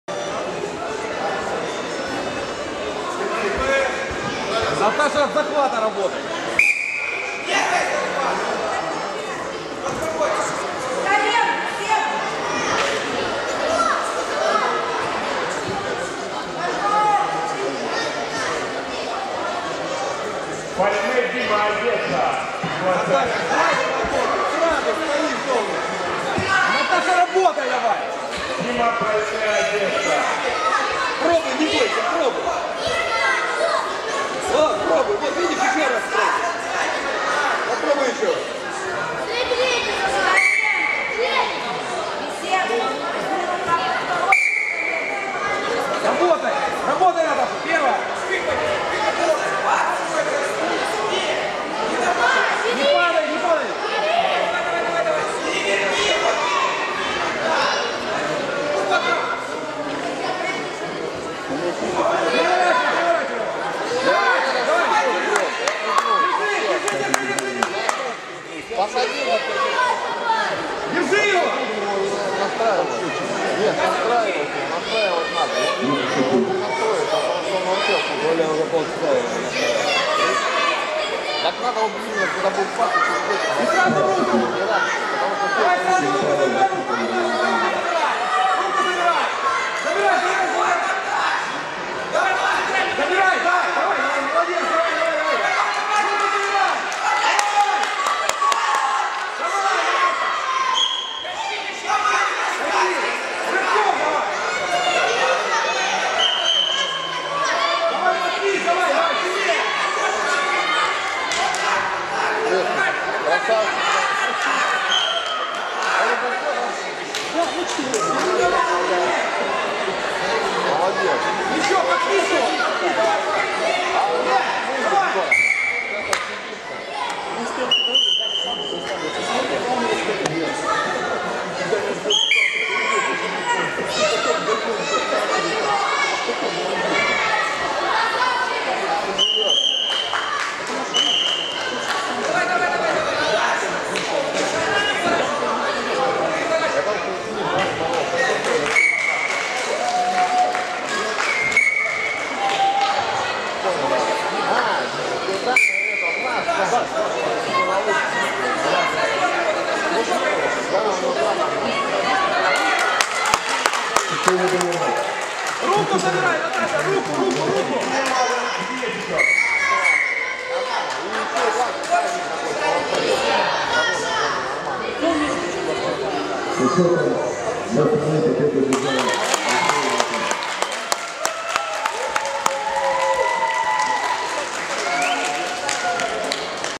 Матвеева Н. самбо 21.10.2012 2 схватка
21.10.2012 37-й традиционный открытый чемпионат города Севастополя по борьбе самбо посвященный памяти Героя Советского Союза Даниила Одинцова среди юношей и девушек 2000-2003 г.г.р.